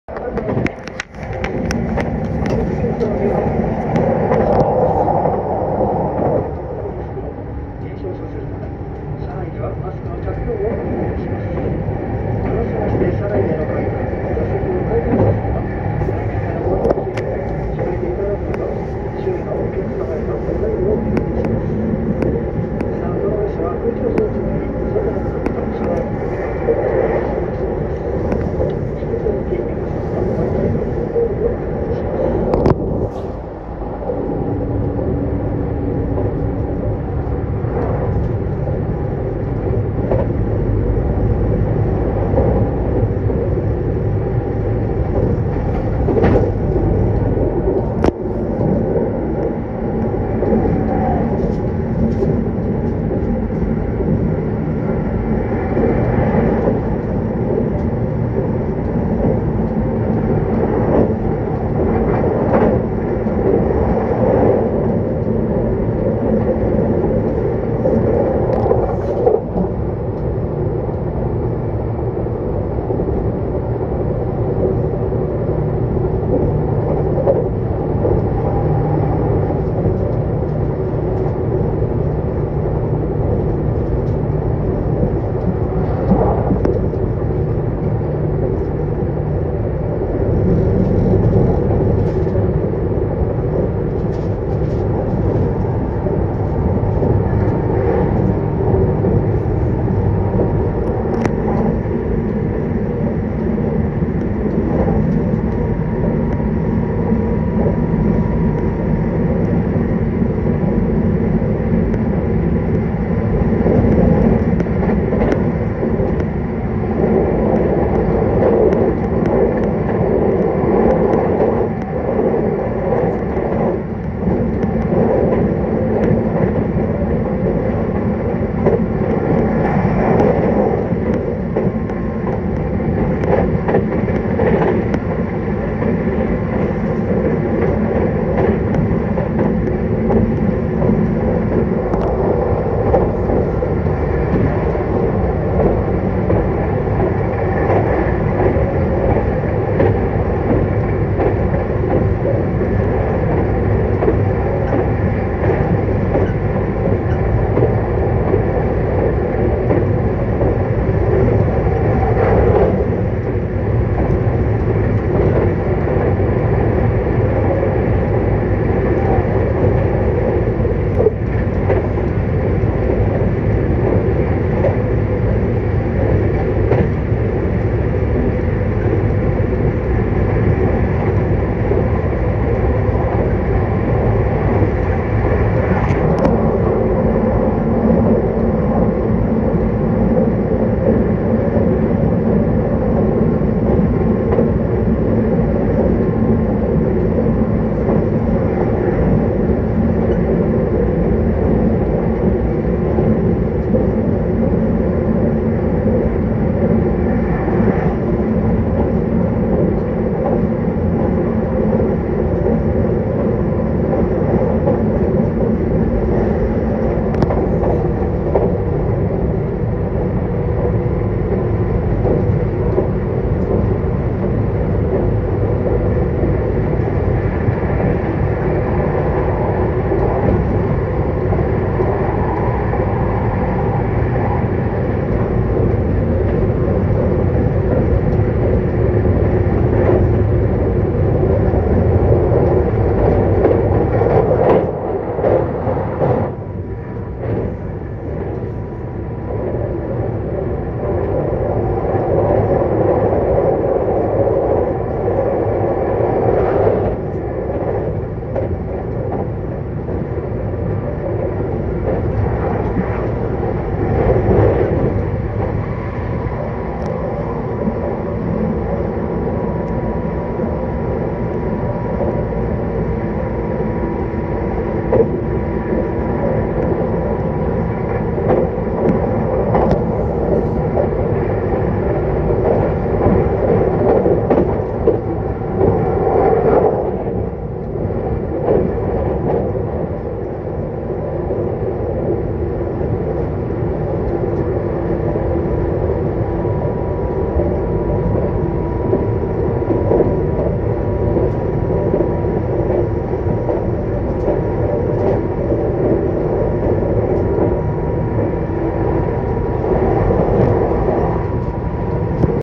旅紀行 さが桜マラソン のコース（ 吉野ケ里 遺跡まで）
列車が佐賀駅を出た直後から、さが桜マラソンで走るコースの方向の風景を、折り返し地点の吉野ケ里遺跡まで撮りました！　のどかな田園風景！